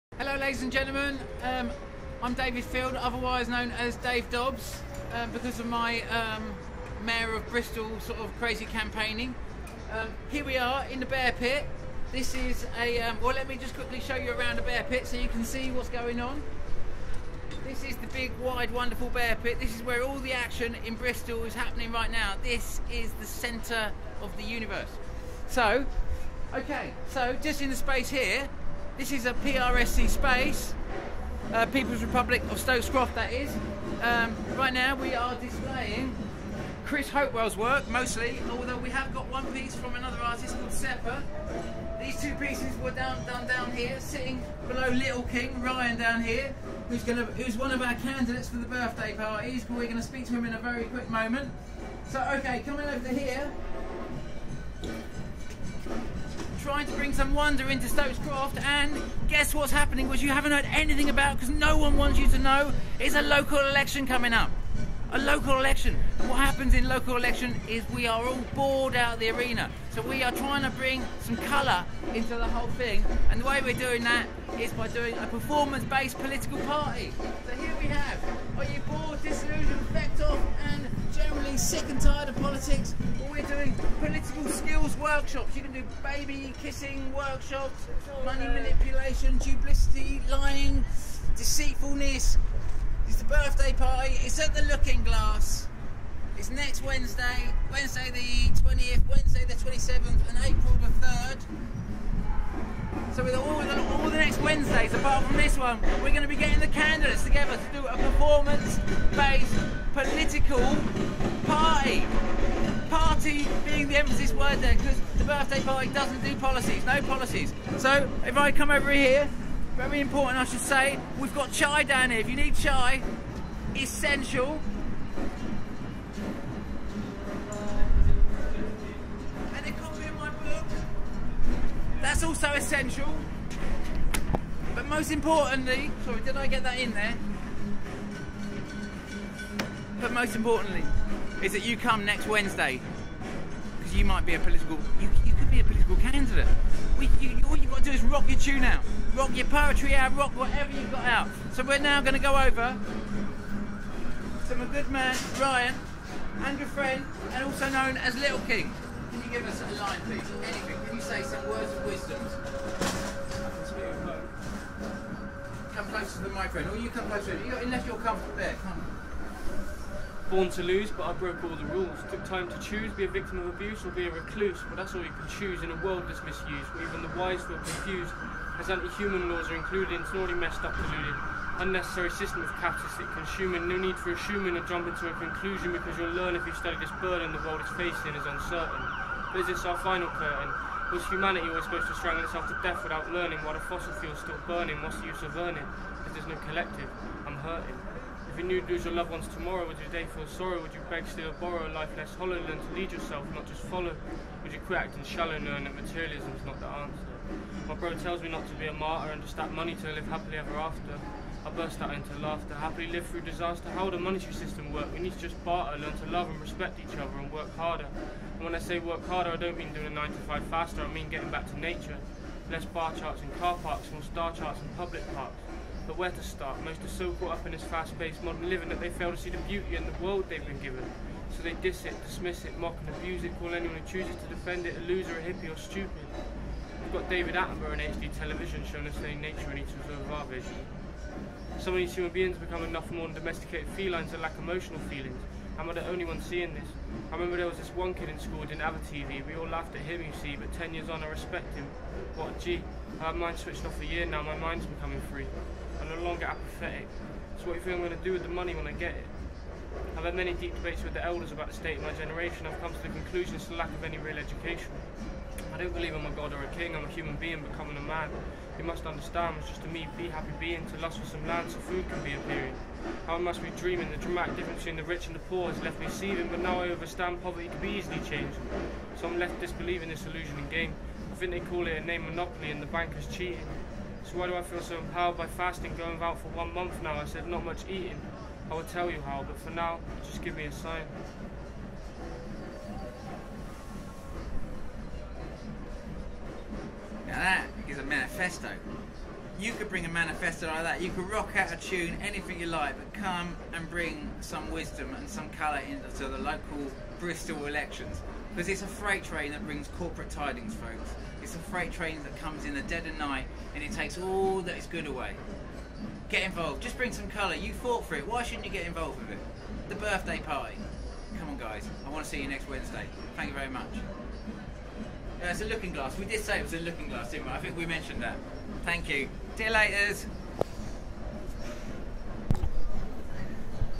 The Birthday Party Hustings Countdown in Bear Pit, Bristol
Dave Dobbs with Little King, Bristol urban vocalist take you into the Birthday Party Hustings countdown taking place on the last two Wednesdays of March (20th and 27th) and the first Wednesday of April (3rd) to get the candidates to run for the Birthday Party across Bristol in the coming local elections. Get involved.